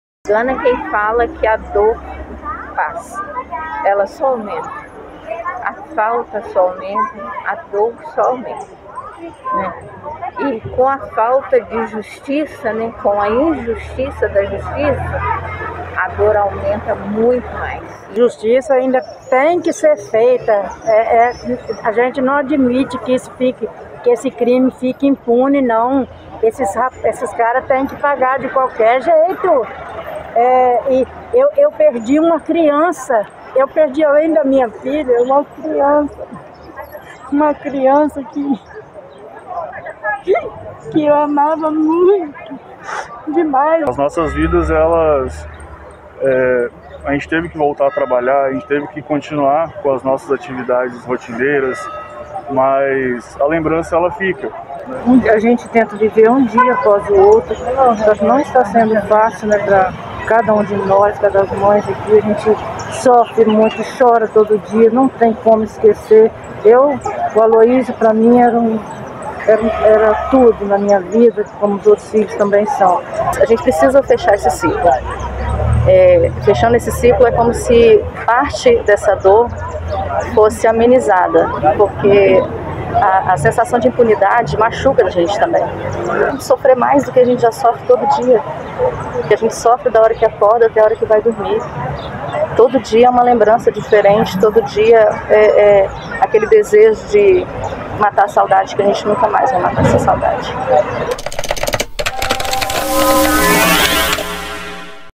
Carreata em homenagem a grupo folclórico é marcada por pedidos de justiça
Acesse O seu portal de notícias!

Faça a notícia junto com o Montanhas Capixabas. Mande agora seu texto, áudio, foto ou vídeo!